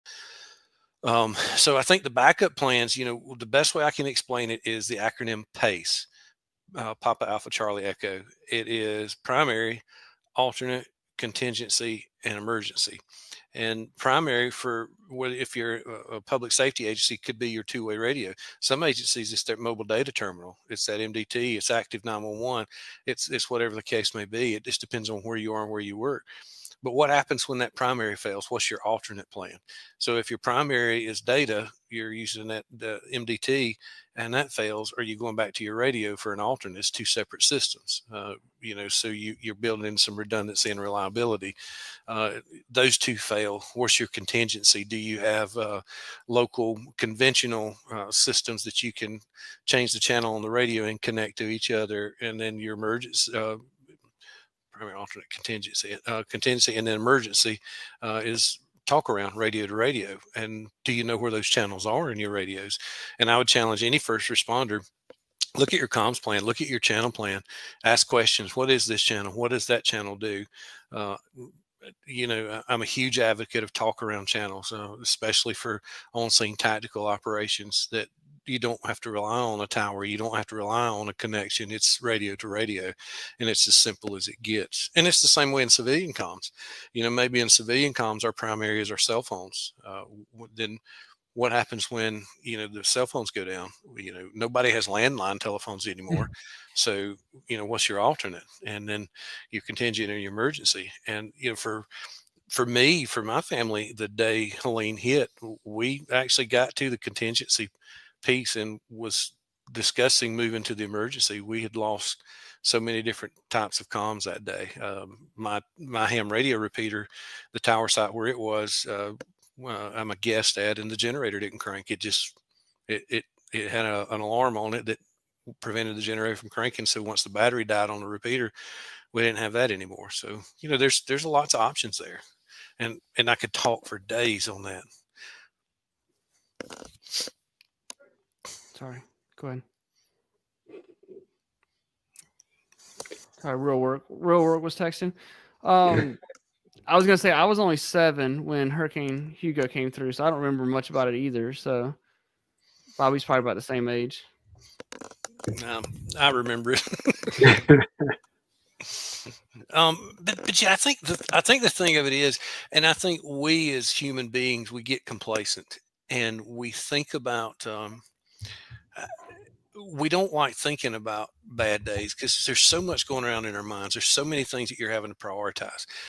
Emergency Comms: PACE Backup Plans for First Responders
We explore PACE (Primary, Alternate, Contingency, Emergency) for comms backup. What happens when primary comms fail? We discuss redundancy, talk-around channels, and real-world scenarios, highlighting the importance of preparedness.